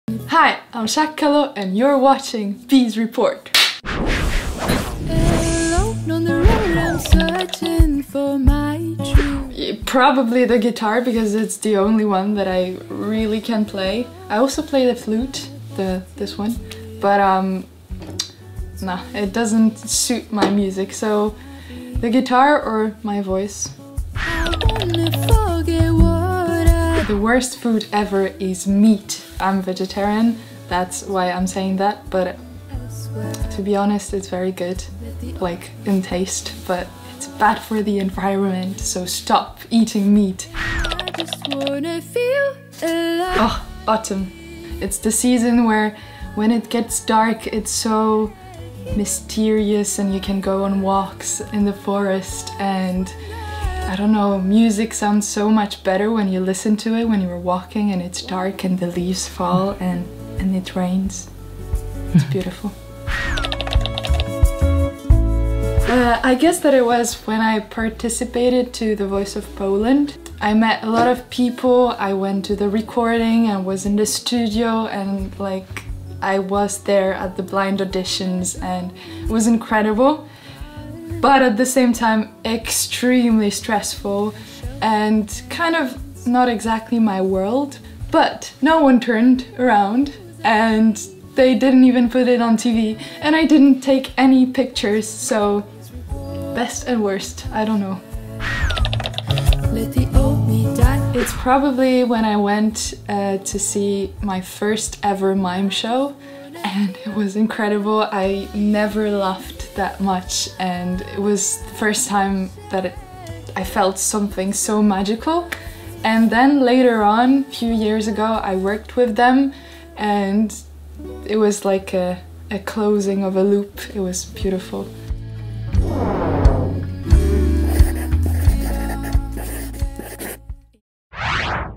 Shakkalo - Best and Worst (INTERVIEW)
Hello ! Voici l'interview "Best and Worst" avec la chanteuse Shakkalo! En espérant que ça va vous plaire !